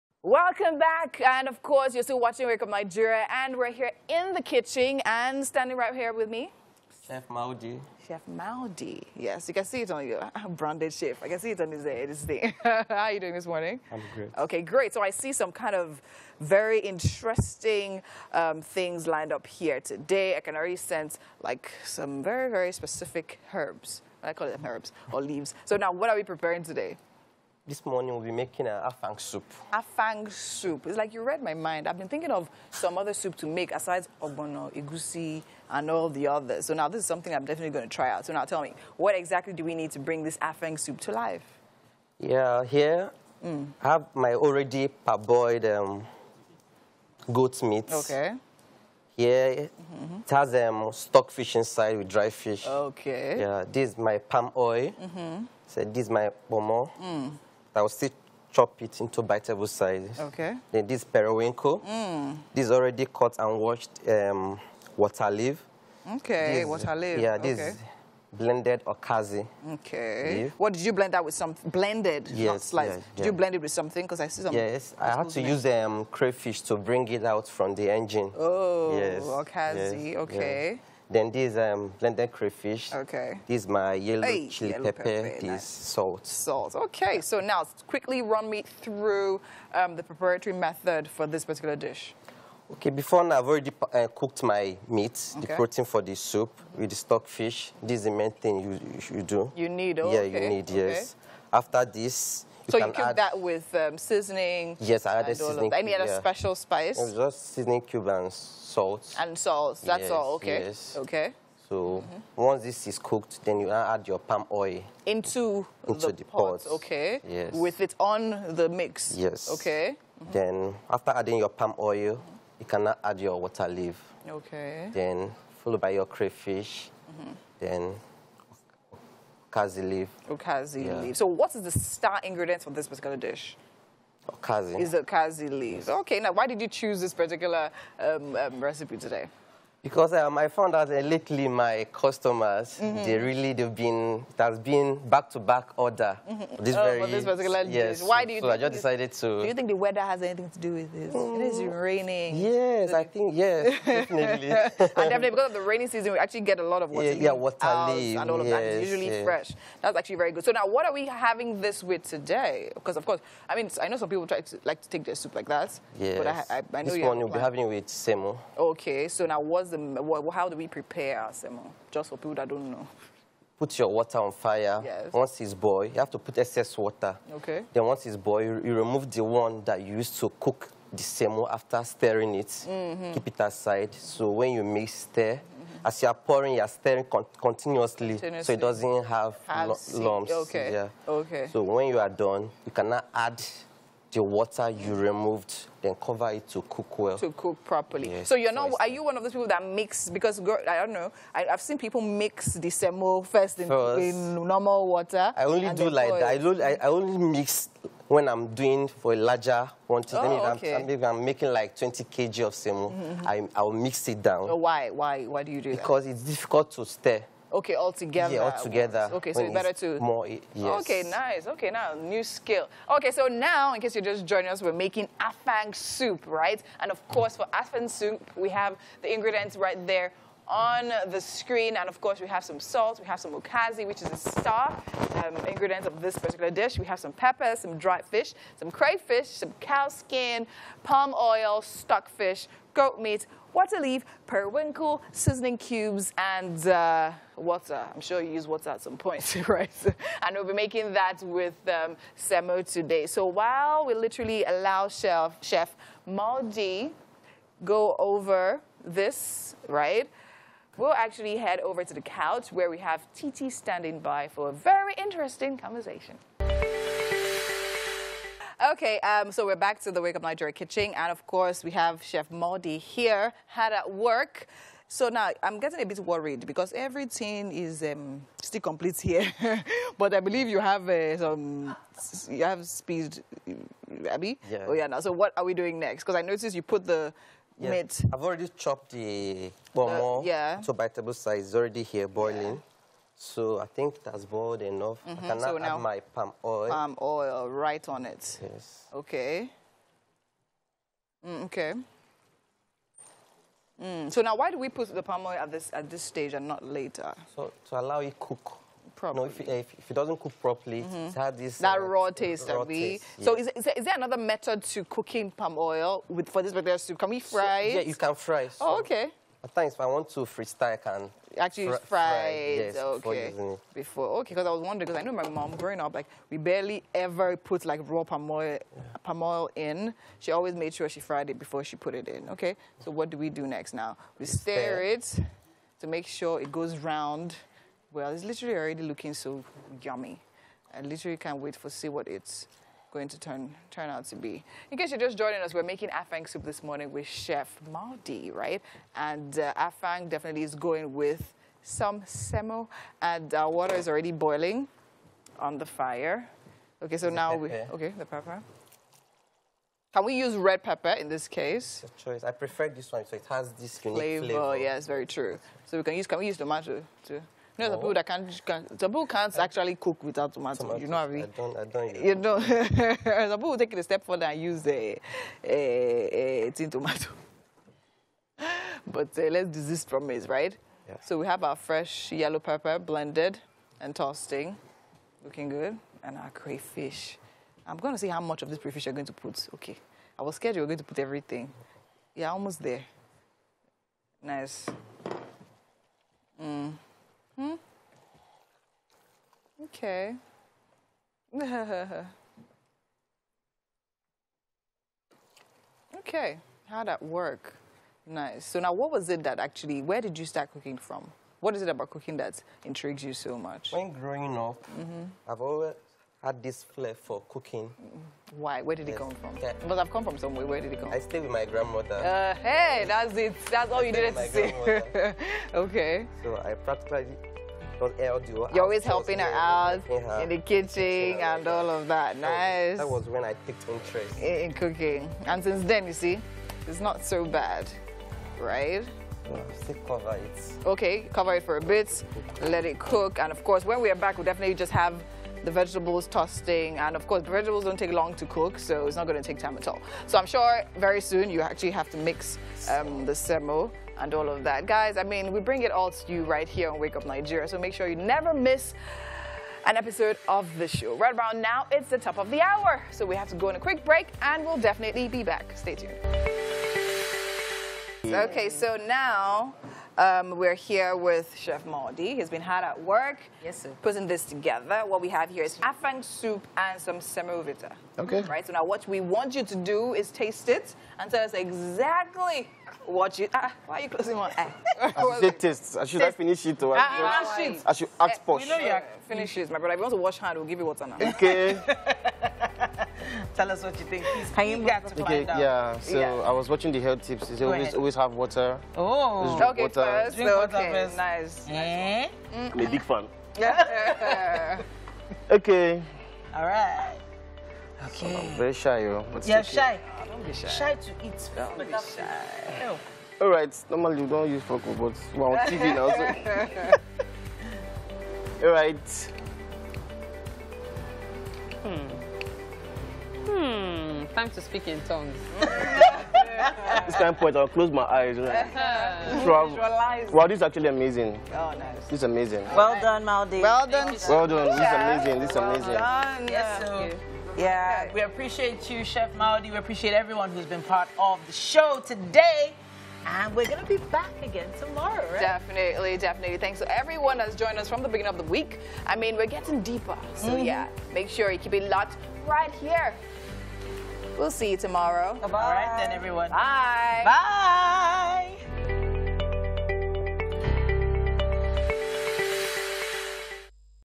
The Best Afang Soup Recipe | Easy and Simple
Chef Maudi prepares his special afang soup and semo in the WUN kitchen.

This is the best Afang soup recipe you will ever come across 😉